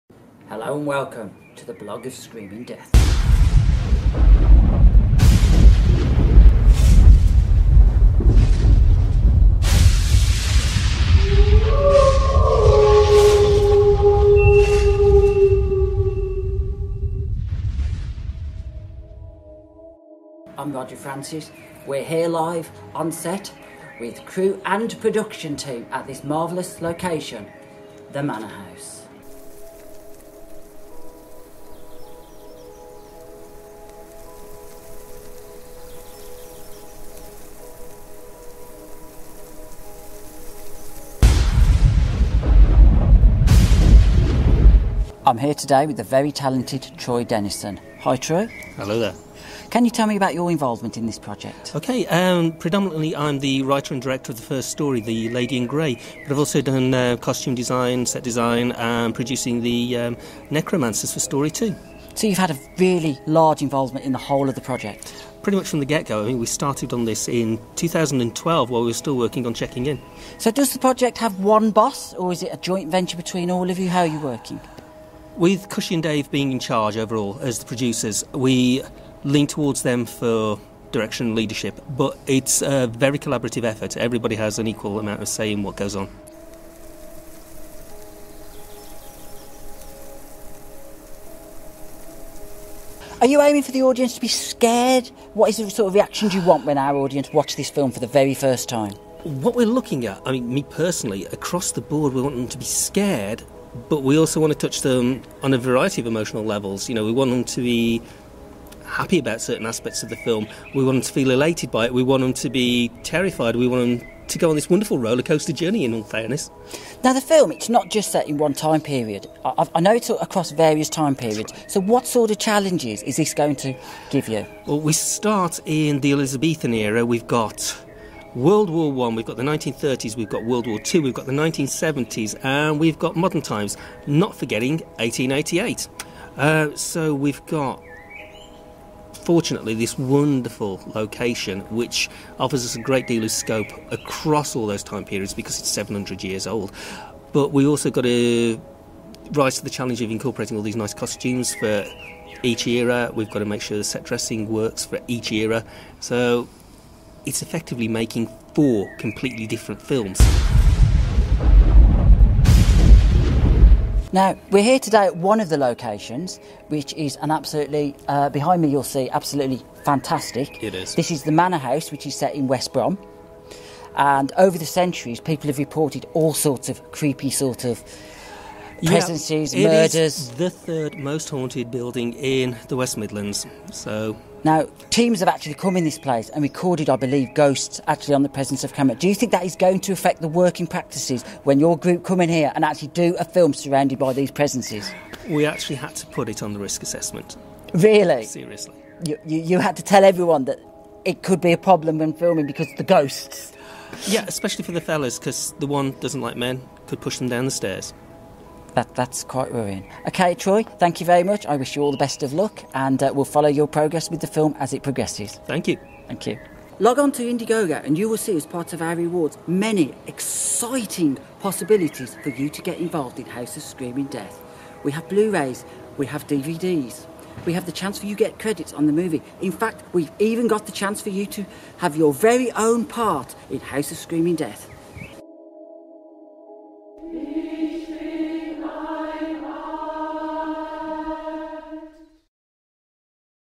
The Blog of Screaming Death 2
The second of our behind-the-scenes featurettes following the progress of "The House of Screaming Death".
Hosted by Roger Francis, in this instalment we visit our wonderfull main shooting location and interview writer/director Troy Dennison

 The House of Screaming Death is currently crowd-funding on Indiegogo, If you can donate then please do at -

and if not then why not follow us on -